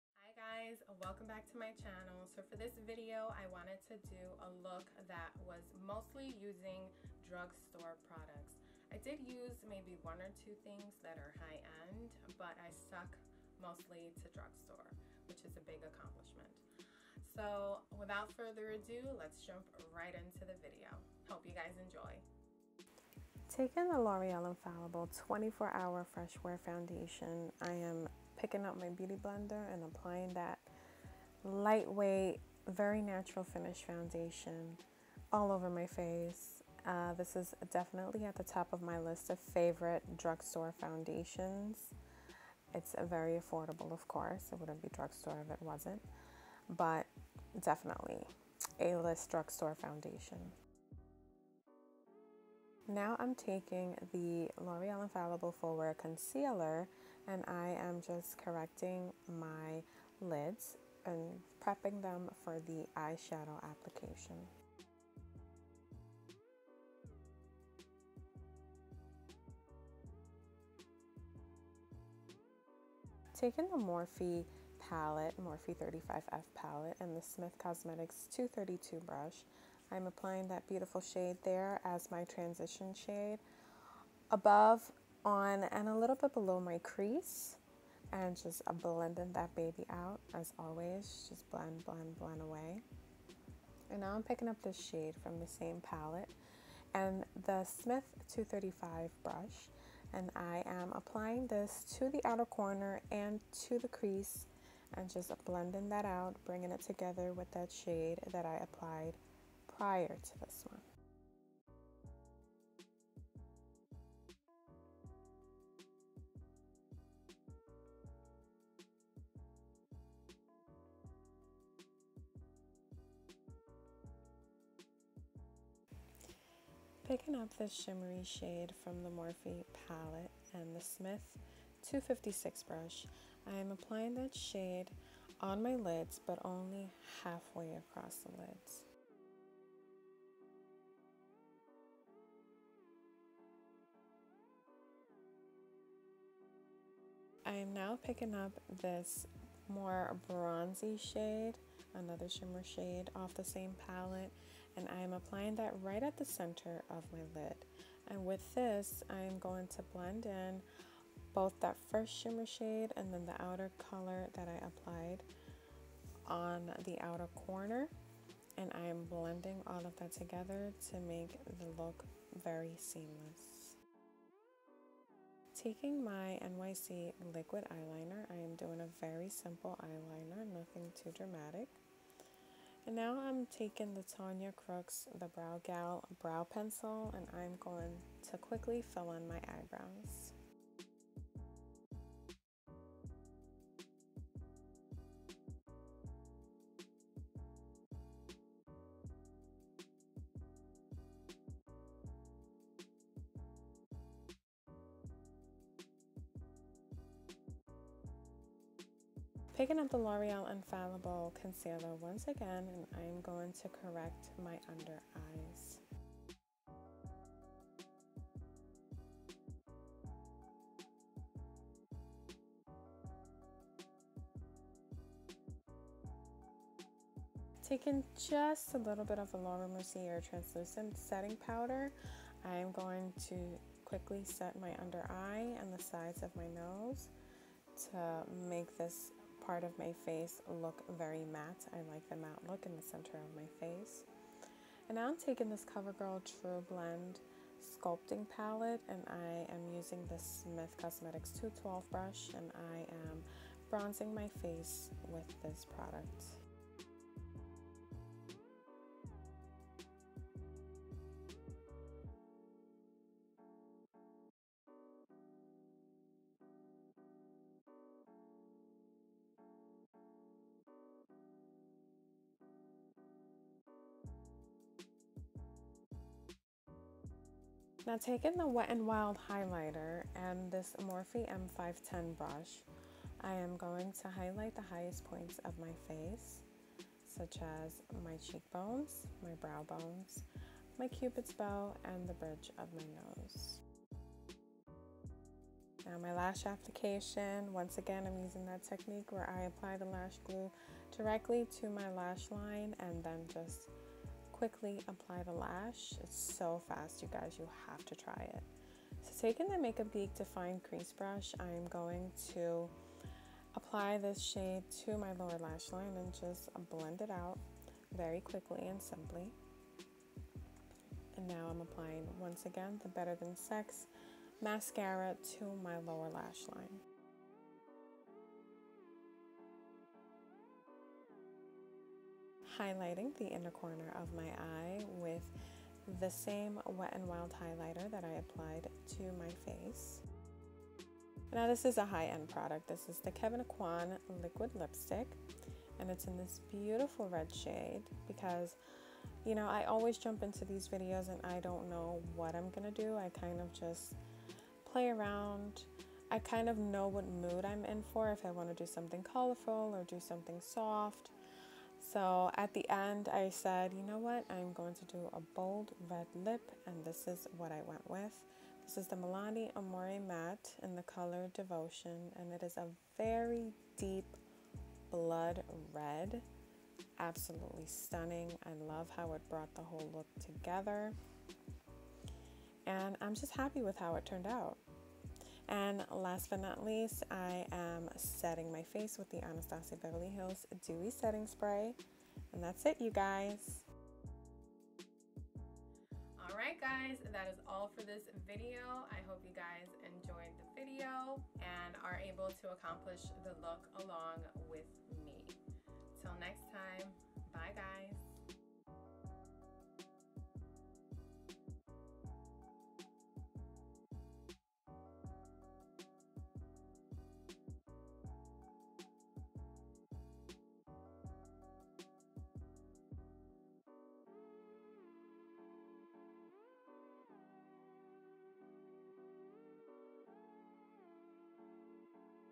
FULL FACE OF DRUGSTORE PRODUCTS!
Hey Guys!

Here's a good video if you're interested in using only drugstore products. MOST of the products I use are drugstore favorites of mine.

"Sui Generis" is Latin for "One of a Kind" " Unique"